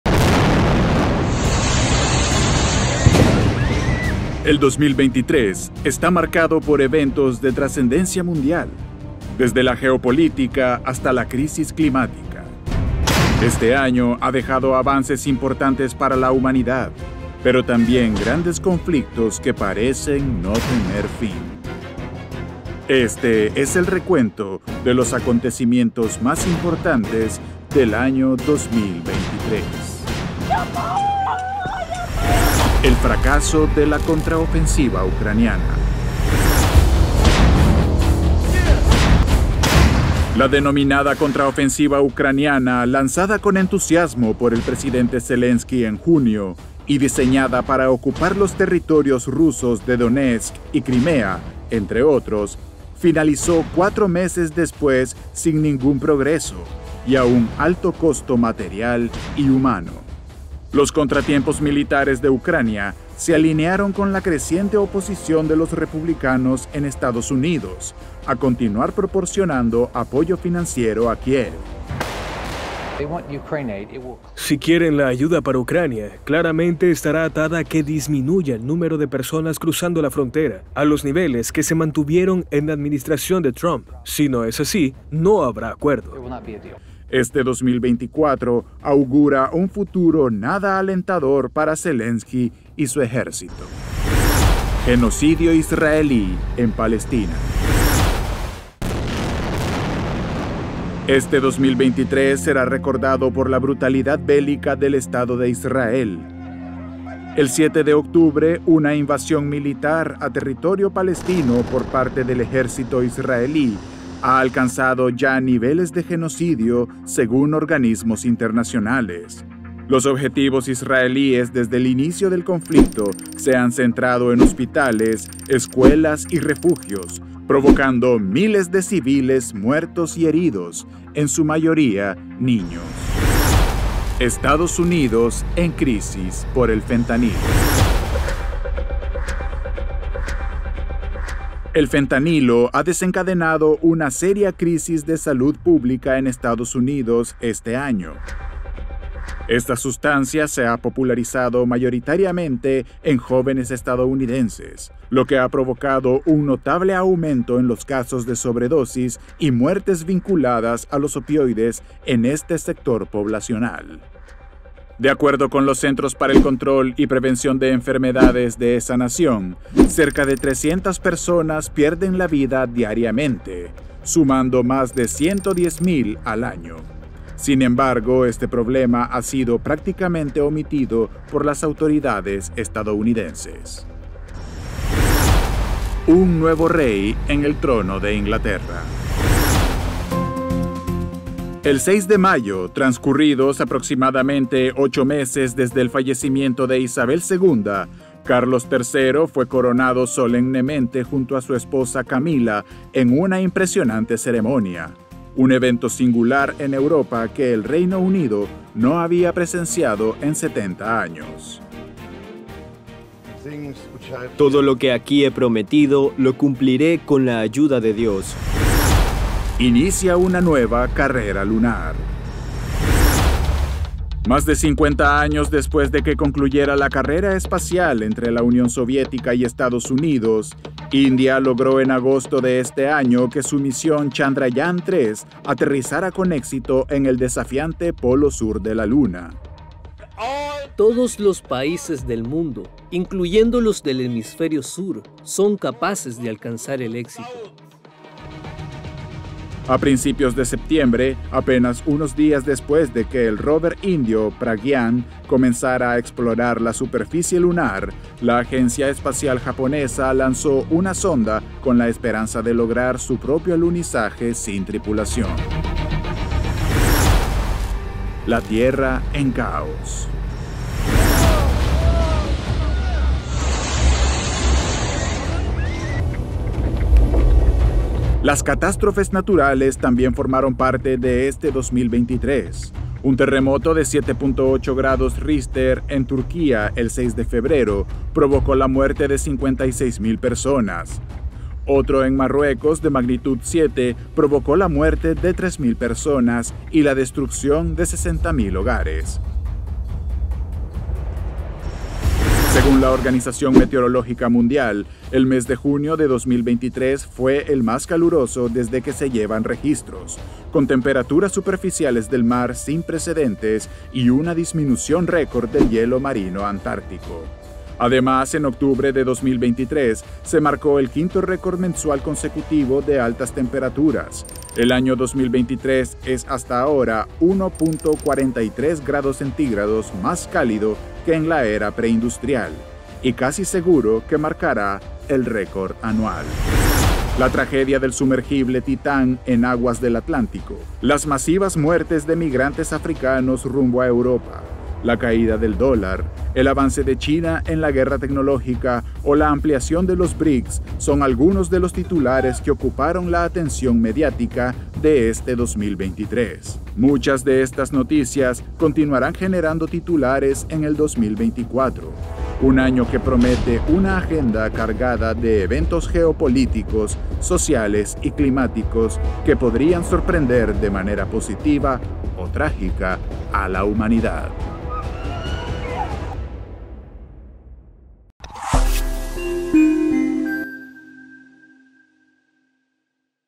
Hechos que marcaron el 2023
❗️ El caos climático, la caída del dólar, el avance de China en la guerra tecnológica o la ampliación de los BRICS, son algunos de los titulares que ocuparon la atención mediática de este año 2023.